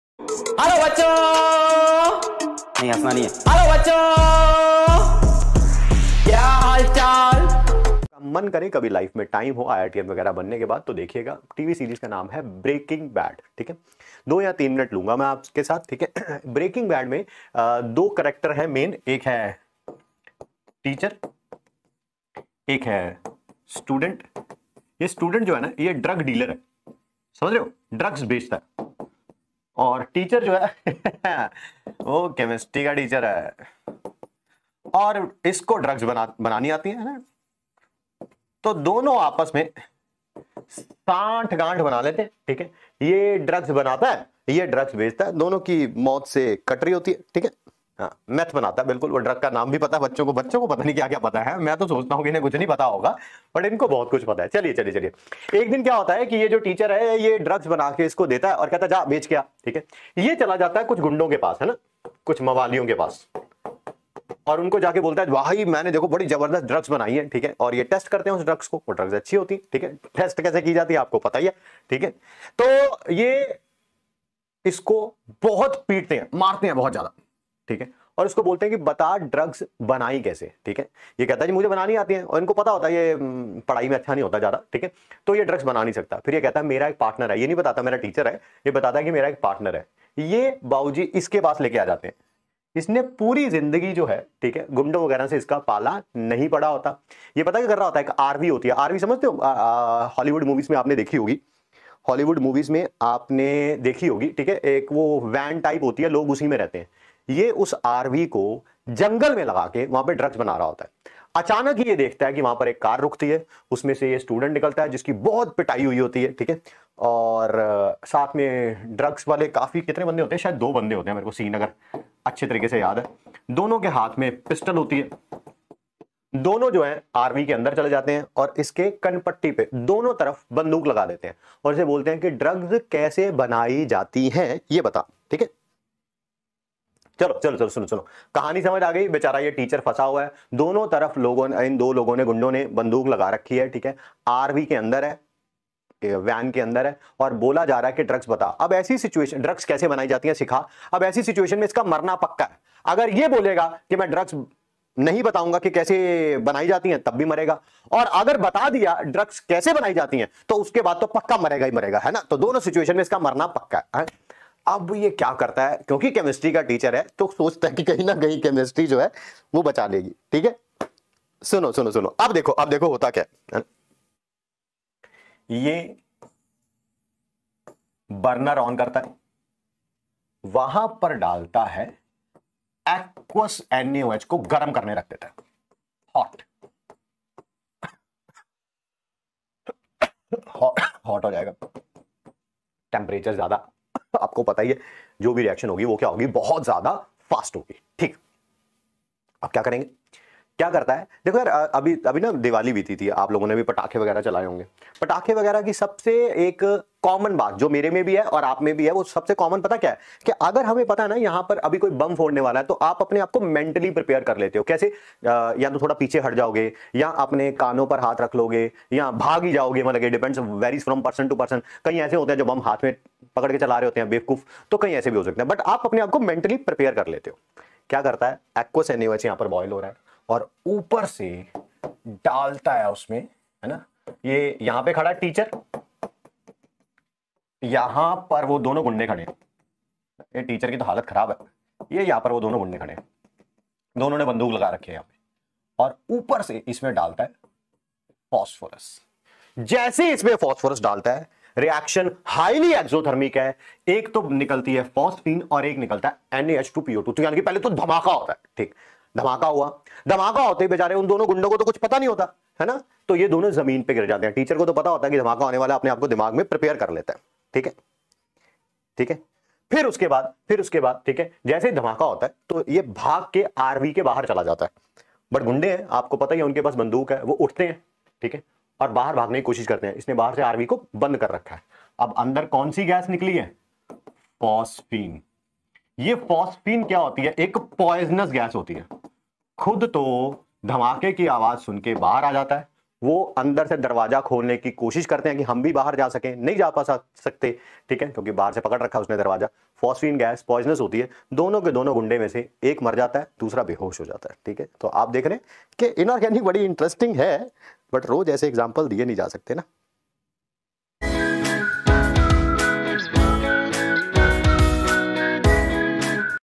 Amit Mahajan Sir telling Hollywood story to Students||Physics wallah||Alakh Pandey||Pw ke Superstar
This video is only for motivational purpose

This video is from wallah's Prayas Batch
in which Amit Mahajan Sir best Chemistry faculty telling a Hollywood story to Students
motivational story
Amit Mahajan Sir
Best motivational quotes
Funny Amit Mahajan Sir
Amit Mahajan Sir looks like Rajkumar Rao